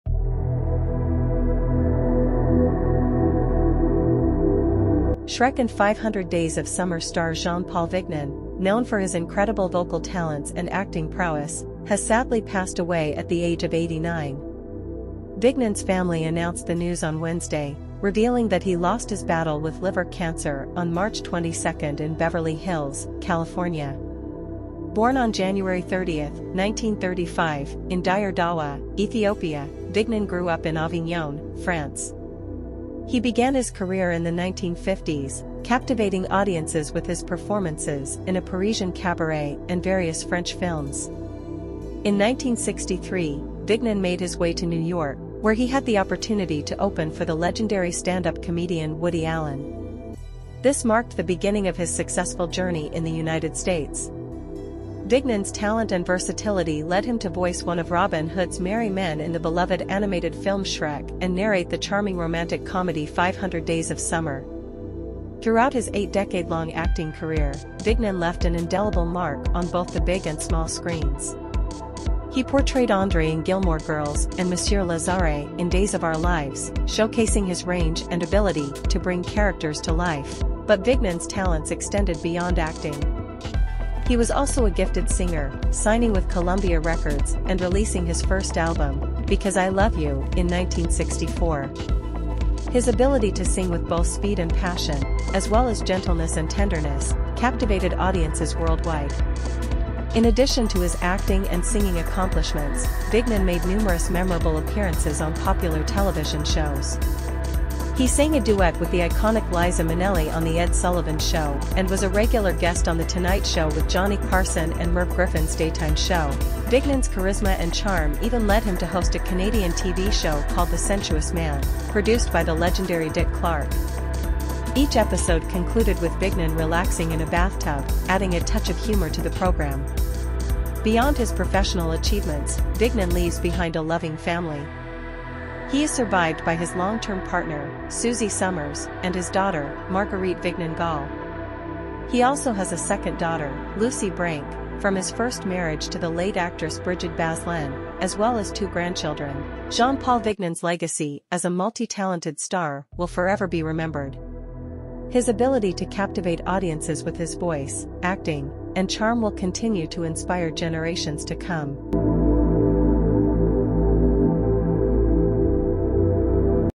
"Legendary Actor Jean-Paul Vignon Dies at 89 After Cancer Battle"
Join us as we remember the incredible talent and legacy of Jean-Paul Vignon, the star of SHREK and 500 Days of Summer. From his captivating voice to his versatile acting, Vignon left an indelible mark on the entertainment industry. Discover the highlights of his career and the impact he made on both the big and small screens.